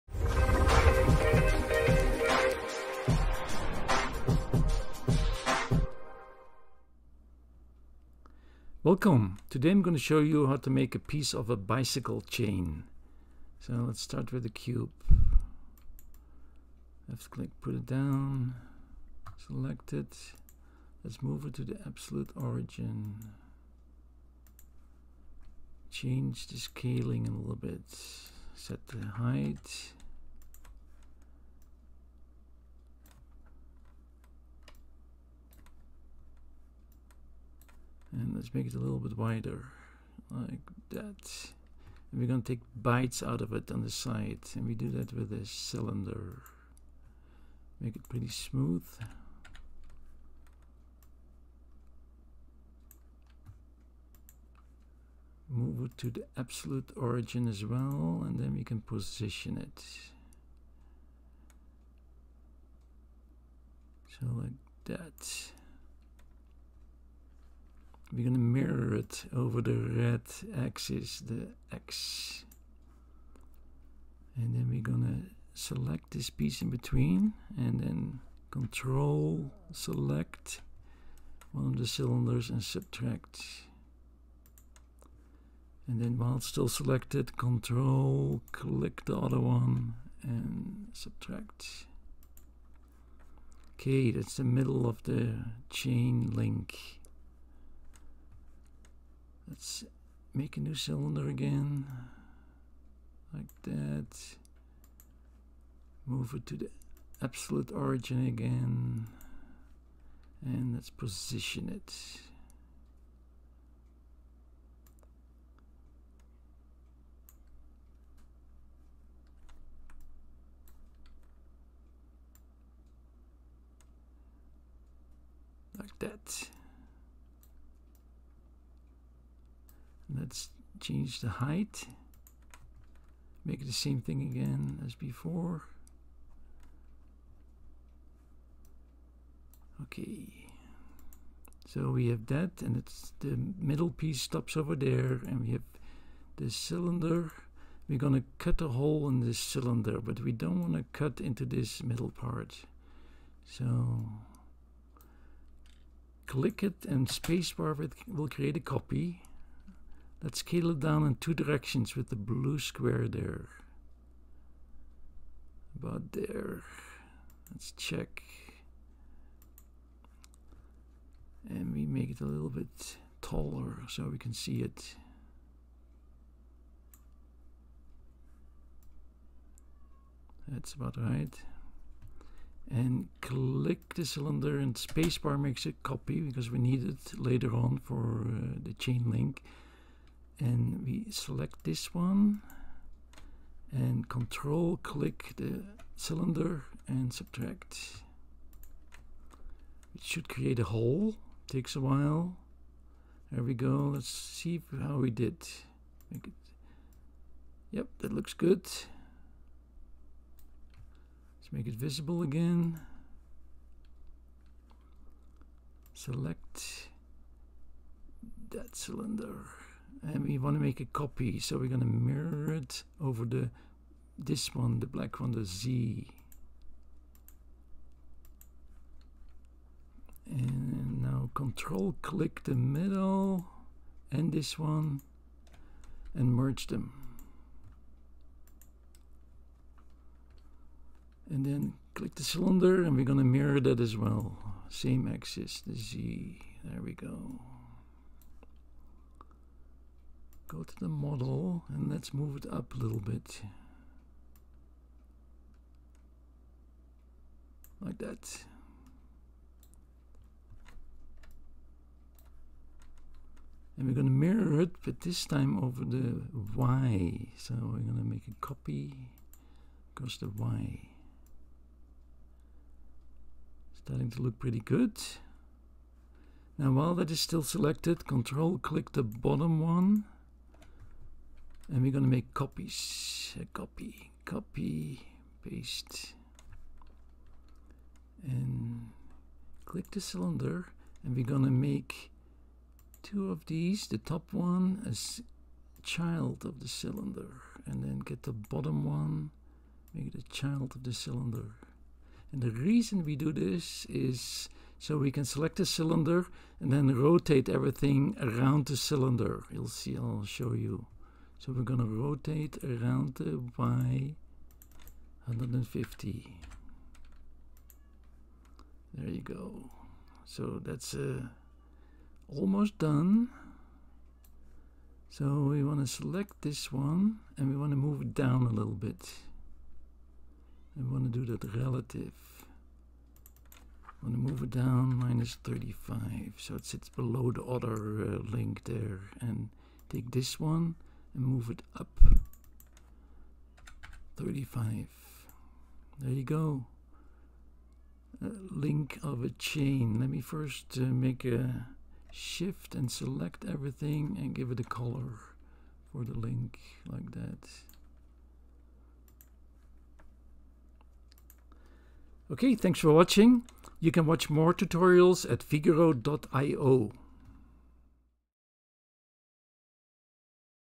Figuro: how to create a 3d bicycle chain.
This video will show you how to create a 3d bicycle chain from scratch in Figuro.